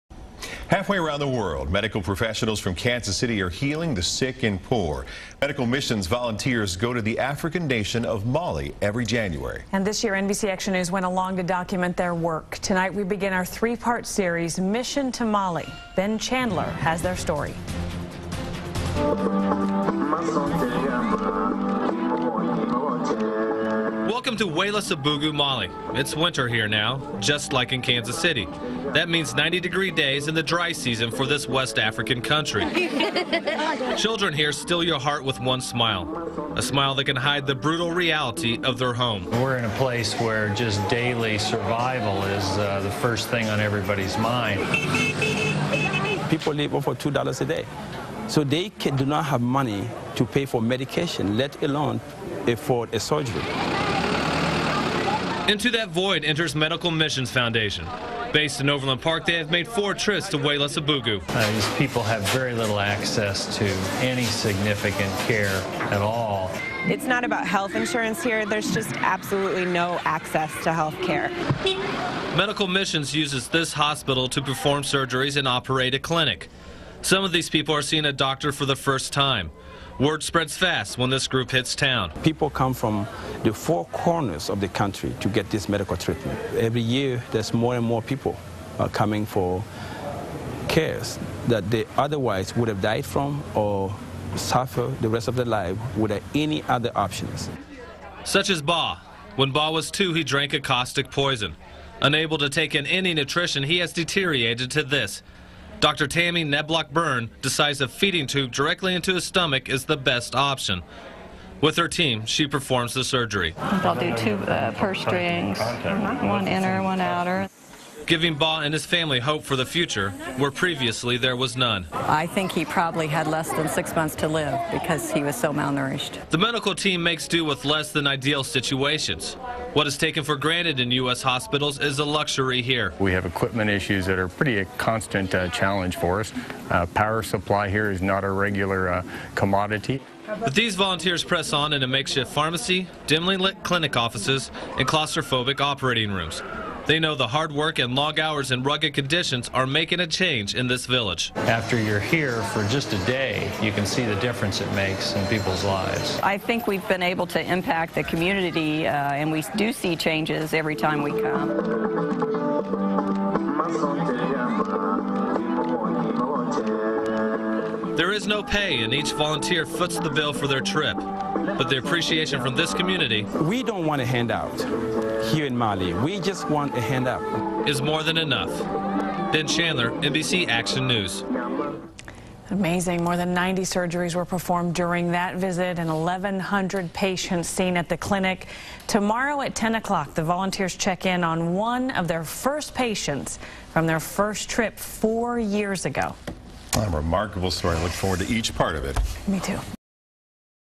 Local mission volunteers go to Mali
Halfway around the world, medical professionals from Kansas City are healing the sick and poor.